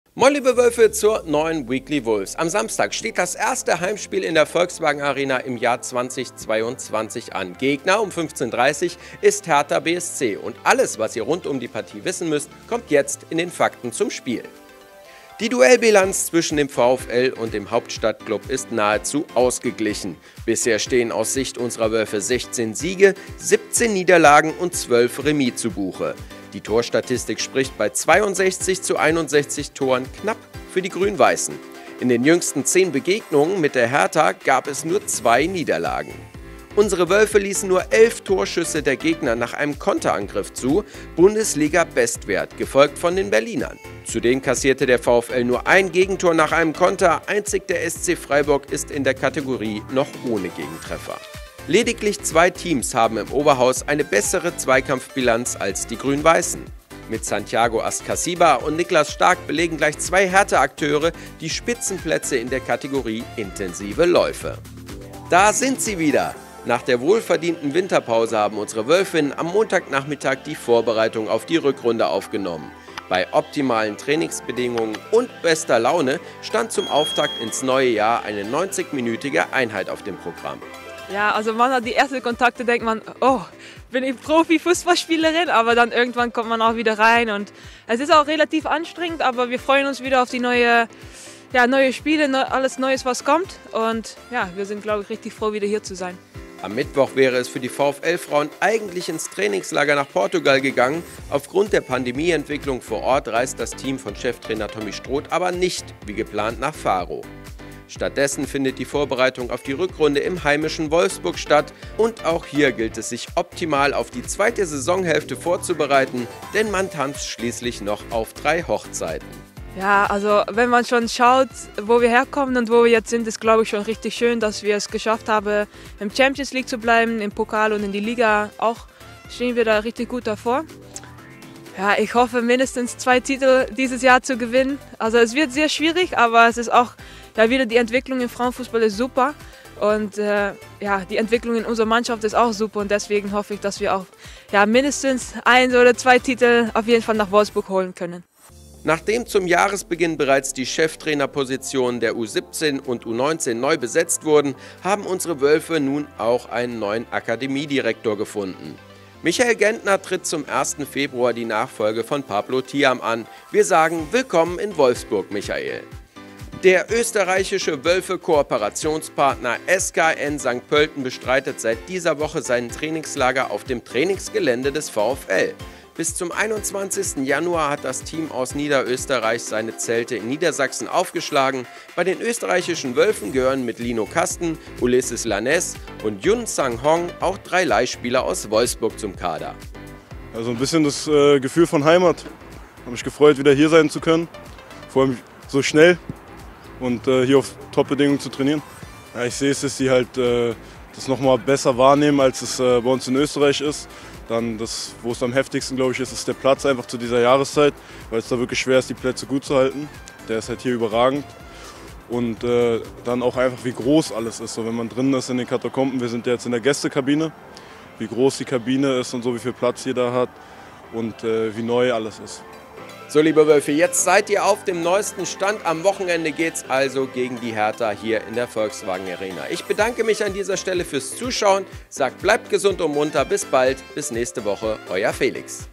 Wölfe treffen auf Hertha - Partnerklub gastiert in Wolfsburg / Wölfinnen legen los! | Weekly Wolves
Weiter geht's mit der Bundesliga für die Wölfe: Das erste Heimspiel im Jahr 2022 steht an und wir liefern euch wie immer alle Fakten zu Spiel und Gegner Hertha BSC. Anstoß ist am Samstag um 15:30 Uhr. Dazu starten auch die Wölfinnen endlich wieder aus der Winterpause mit der Vorbereitung für die Rückrunde und es gibt neues von der Akademie. Viel Spaß mit der neuen Folge der Weekly Wolves!

Alle Themen im Überblick:
00:00 Intro
00:14 Fakten Hertha BSC Berlin
01:04 Wölfinnen
02:28 News
03:41 Auf Geht’s!